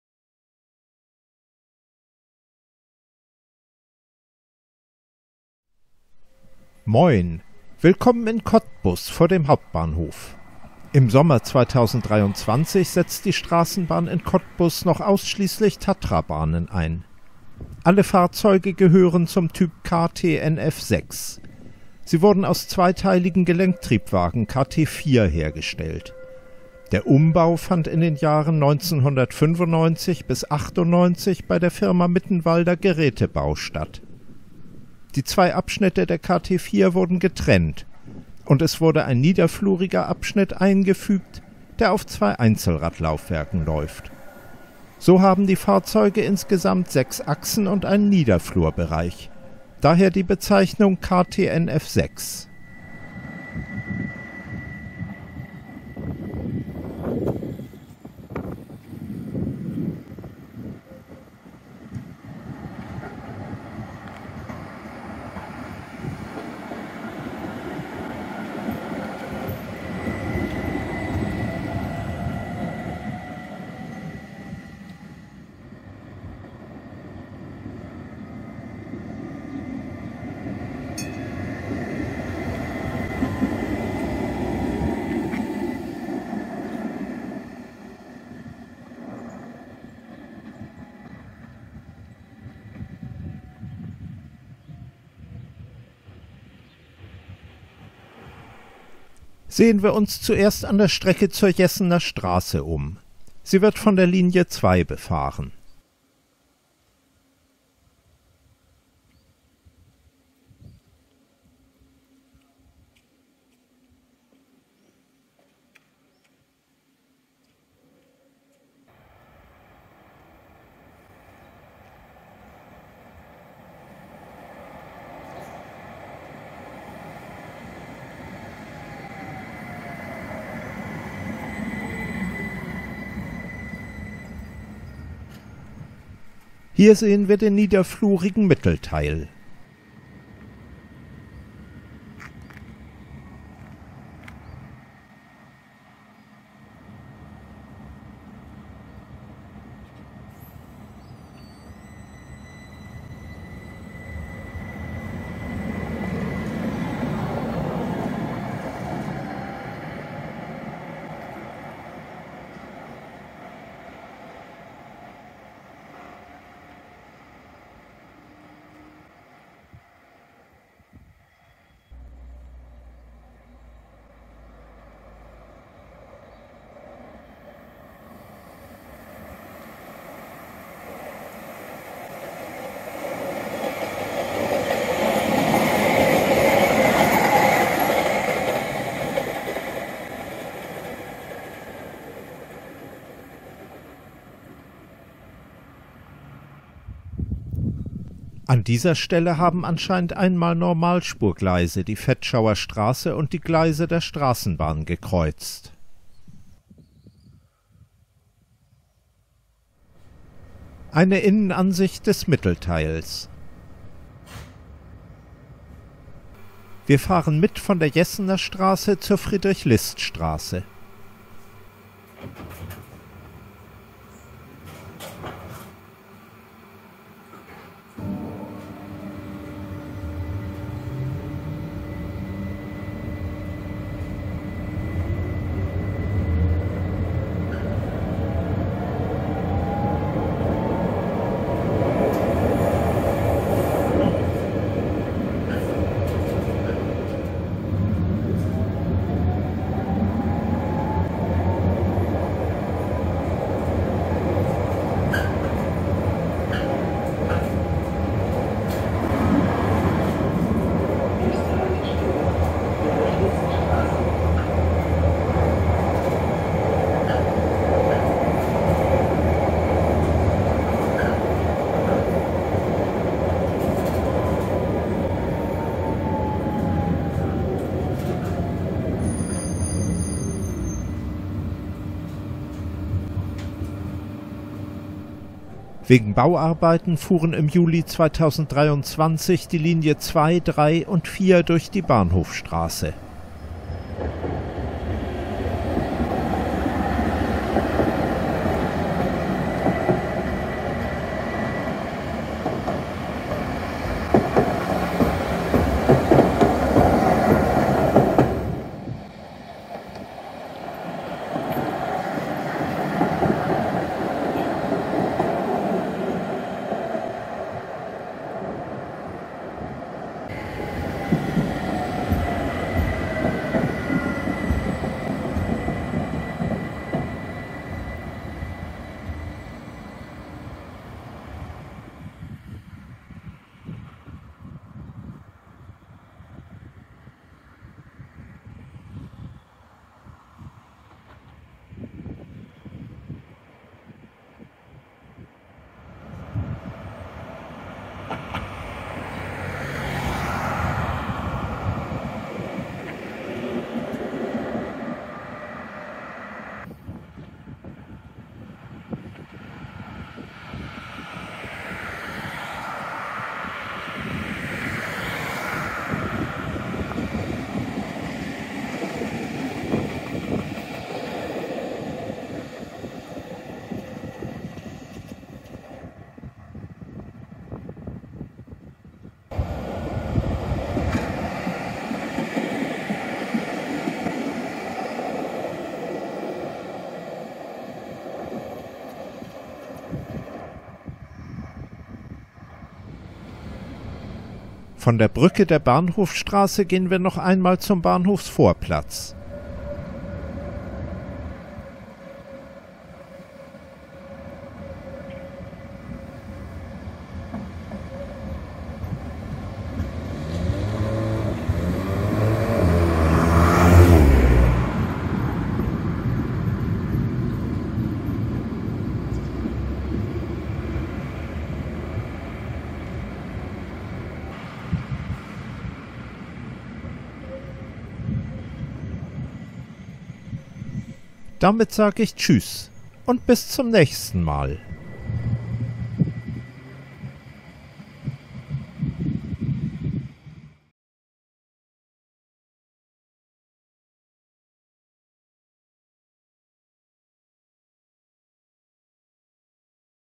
Straßenbahnen – Trams in Cottbus im Sommer 2023 – Betrieb mit Tatra-Bahnen KTNF6
English subtitles available, English description below – Im Sommer 2023 setzt die Straßenbahn in Cottbus (Cottbusverkehr) noch ausschließlich Tatra-Bahnen ein. Alle Fahrzeuge gehören zum Typ KTNF6. Sie wurden aus zweiteiligen Gelenktriebwagen KT4 hergestellt. Der Umbau fand in den Jahren 1995–1998 bei der Firma Mittenwalder Gerätebau statt. Die zwei Abschnitte der KT4 wurden getrennt, und es wurde ein niederfluriger Abschnitt eingefügt, der auf zwei Einzelradlaufwerken läuft. So haben die Fahrzeuge insgesamt 6 Achsen und einen Niederflurbereich – daher die Bezeichnung KTNF6.
Bald wird sich diese Situation ändern. Die Straßenbahnbetriebe von Cottbus, Brandenburg (Havel) und Frankfurt(Oder) – alle drei fahren auf Meterspur – haben gemeinsam dreiteilige Fahrzeuge des Typs ForCity Plus bei Škoda Transportation bestellt. Die Bahnen für Cottbus sollen voraussichtlich zwischen 2024 und 2026 geliefert werden.
Wegen Bauarbeiten fuhren im Juli 2023 die Linie 2, 3 und 4 durch die Bahnhofstraße.

In summer of 2023 Cottbusverkehr, the tram operator of Cottbus, uses Tatra trams exclusively. All vehicles are of the type KTNF6. They were rebuilt from 2-section articulated vehicles of type KT4. Reconstruction took place at the firm Mittenwalder Gerätebau during the years 1995–1998. The 2 sections of KT4 were separated, and a low floor section was inserted, running on 2 single axle chassis with separate wheels. So the reconstructed vehicles have 6 axles and a low-floor section. That's why they are called KTNF6 (KT Kurzgelenk-Triebwagen = short articulated tramcar, NF Niederflur = low-floor).
This situation will change soon. The tram companies of Cottbus, Brandenburg (Havel) und Frankfurt(Oder) – all three of them running on 1000 mm gauge –have ordered  together 3-section vehicles of type ForCity from Škoda Transportation. Trams for Cottbus are expected to be delivered between 2024 and 2026.
Due to construction works, lines 2, 3, and 4 passed through Bahnhofstraße (station street) in July of 2023.